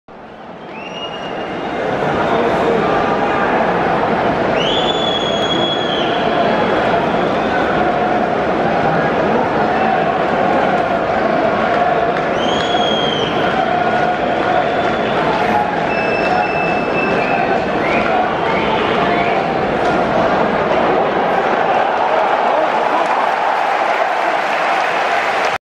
Conversion Kick Romain Ntamack(FRA) ①
RUGBY WORLD CUP JAPAN日本2019
Match 3
France v Argentina
9.21(sat) Tokyo Stadium(Tokyo,Japan)